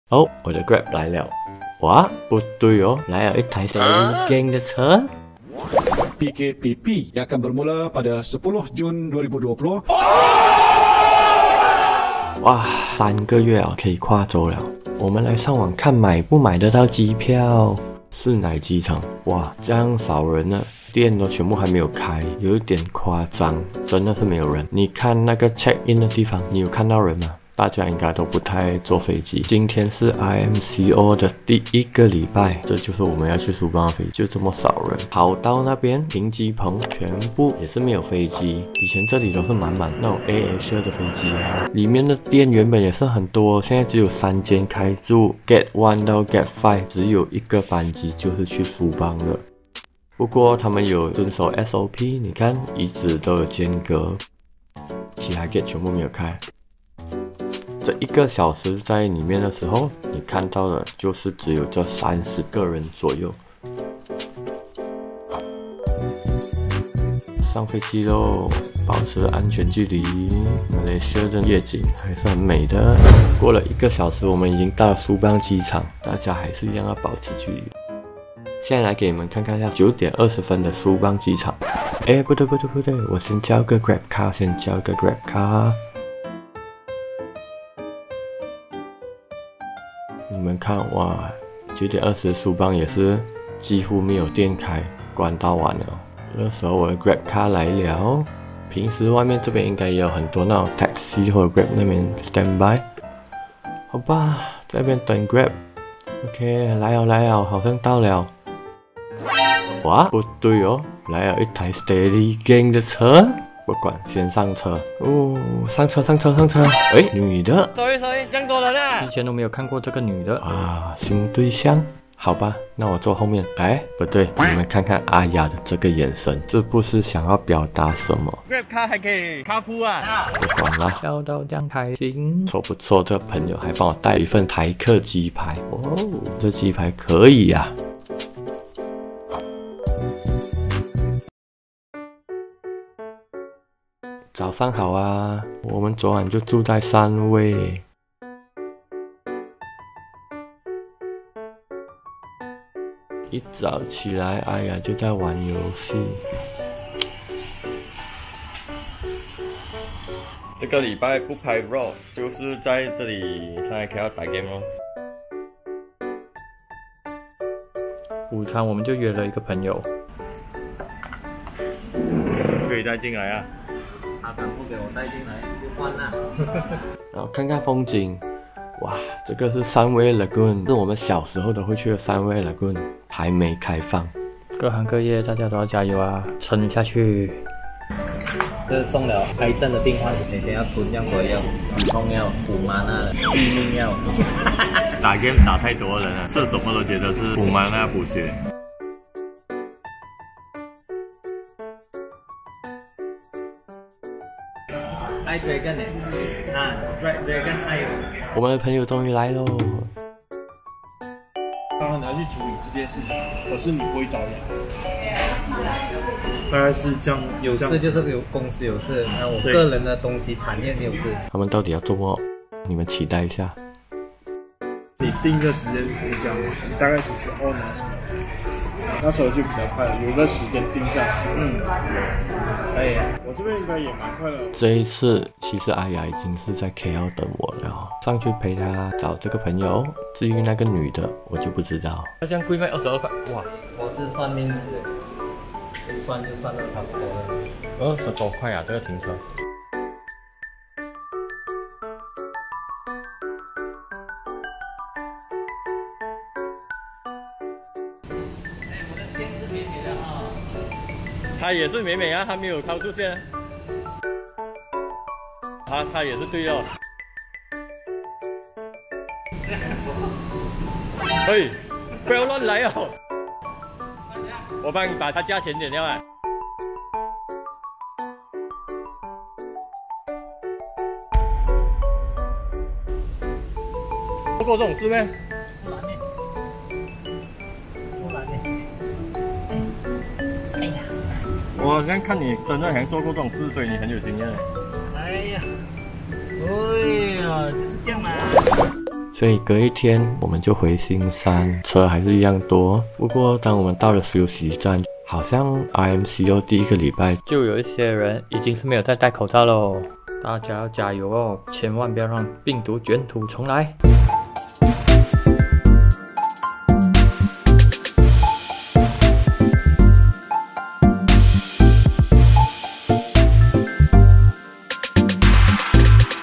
MCO 解封後 的飞机场✈️ | 上KL @AhyaSteadyGang 居然偷偷 | Steady Grab 机场都没人
RMCO 第一周
政府说可以跨州了，我们就帮大家看看
国内飞机的状况，机场的人潮真的少
到了吉隆坡就跟阿亚一起去找朋友

居然有个女人!

Thk You All the BGM support